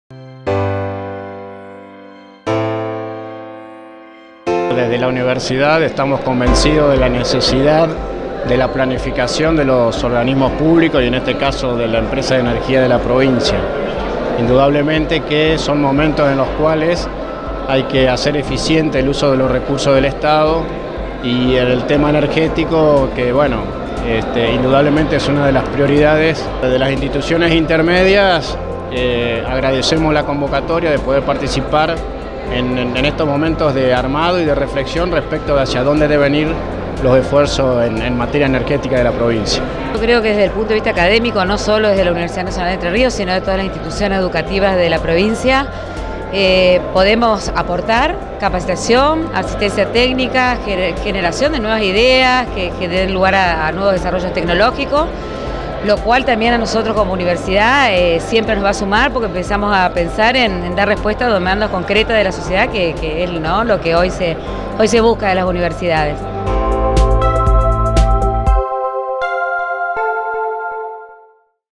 Plan Energético a 10 años. Conclusiones finales. Entrevistas 3era parte
PlanEnergético10Años | Conclusiones finales
Continuamos con las entrevistas realizadas el pasado viernes en el Centro Provincial de Convenciones a Andres Sabella, Decano de la Facultad de Ciencias Económicas UNER y Cra. Silvina Ines Ferreyra, Vicedecana de la Facultad de Ciencias Económicas, UNER, quienes nos brindaron sus opiniones y reflexiones sobre este proyecto energético a 10 años.

Publicado el 16 de noviembre de 2017